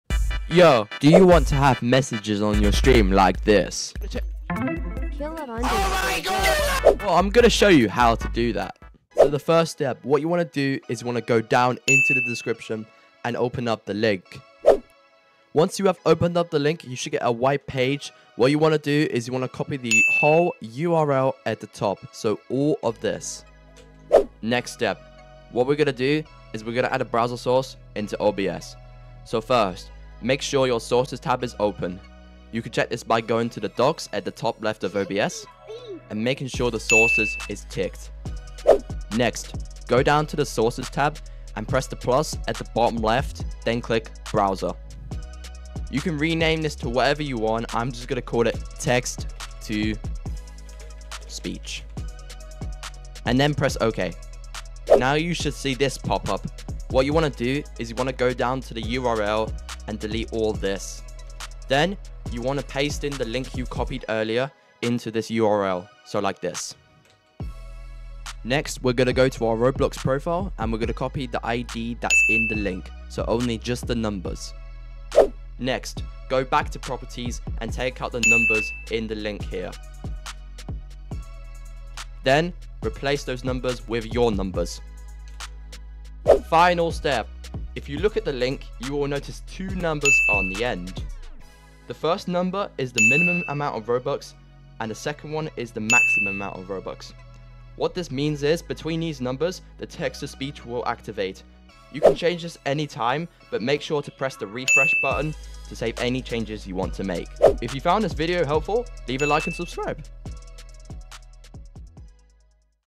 How to Insert TTS into your PLS DONATE STREAMS! - Simple Tutorial!
Today , I'm going to be giving a tutorial on how to insert TTS into your streams with very easy and simple steps!

TIPS FOR TTS USE 👇

Make sure to click the "refresh" button when you..

- Change Numbers in properties.
- Moving the TTS to a different spot in your streams.
- When TTS is displaying, you can also mute any donos by also clicking refresh.

Note : This is for YOUTUBE Streams on OBS ( MAY or MAY not work with streamlabs )

ROBLOX GROUP : ZTA CLAN

Dropping new content soon!

ROBLOX : zT2Dz